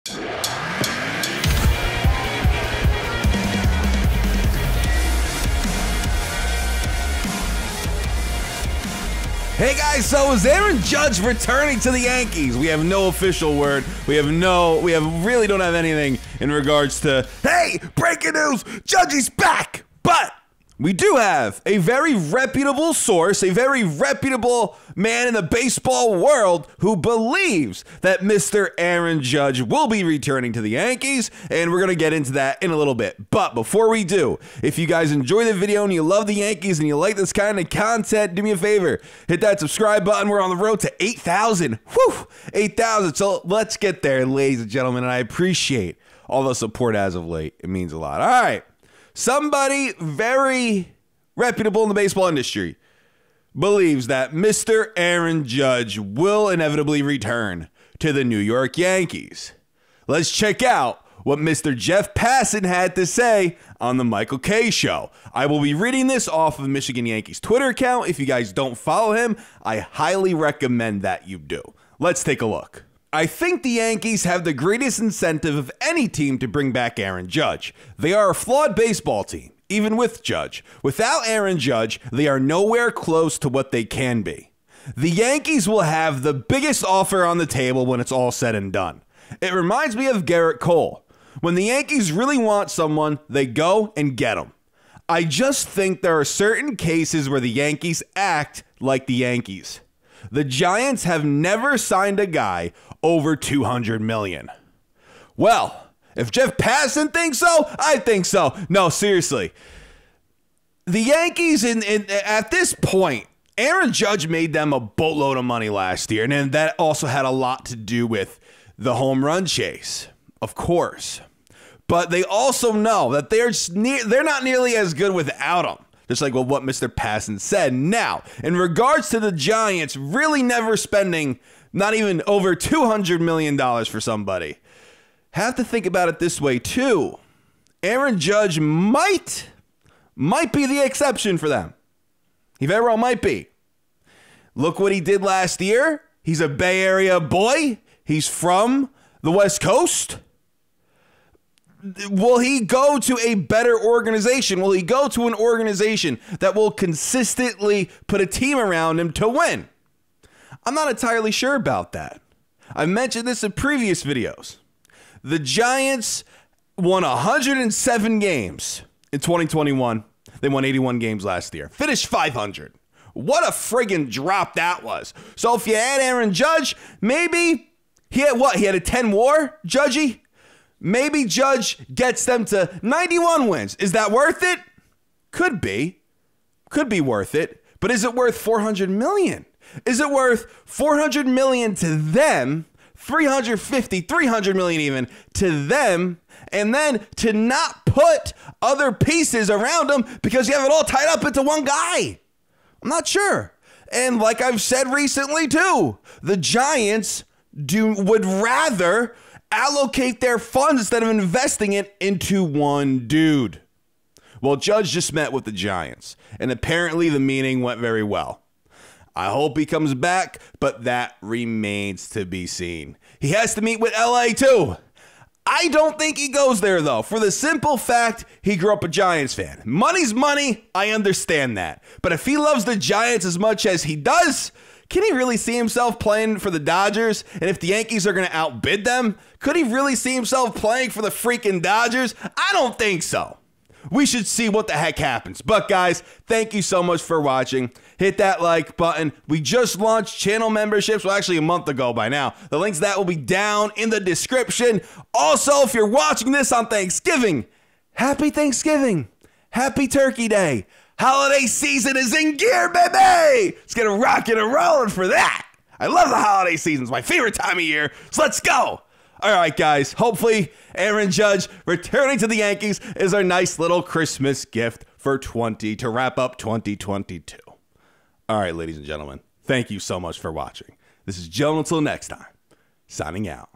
Yankees NEWS: Aaron Judge Return Likely?
Aaron Judge returning to the Yankees? Let me know what you guys think. Take care.